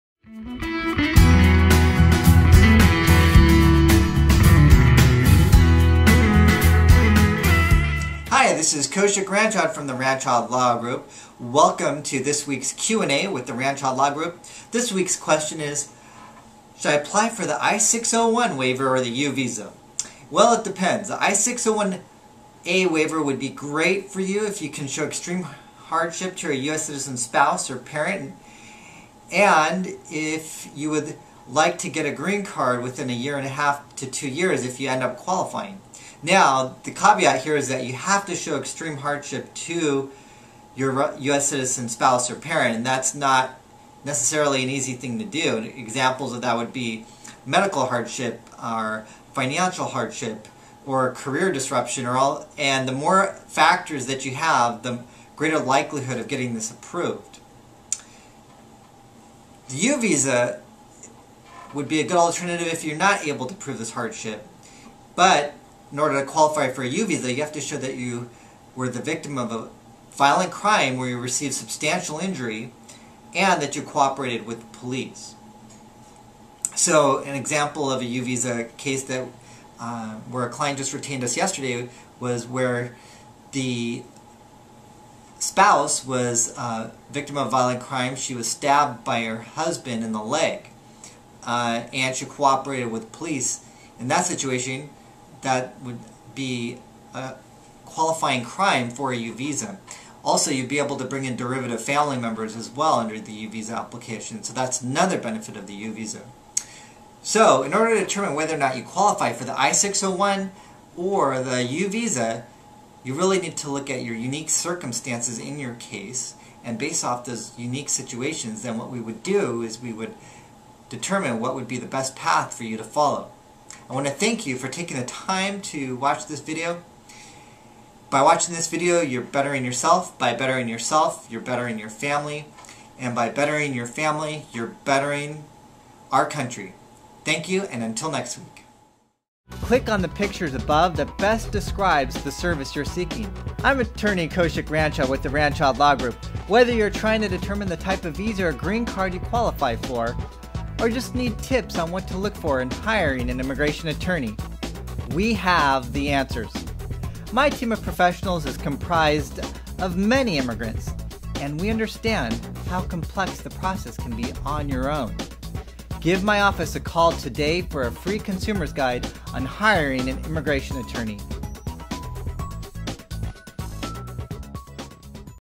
Difference between I601A and U-Visa: Sacramento, San Francisco, Santa Clara
Know whether to apply for the I-601 waiver or U-visa.